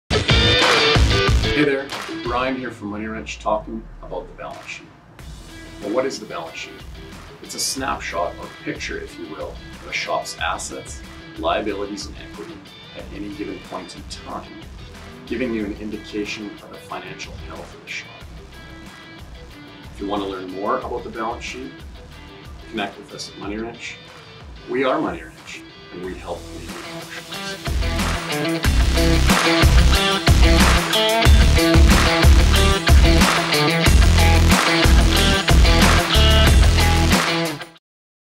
MoneyWrench - What is a balance sheet?
Bryan quickly explains what a balance sheet is and why is matters.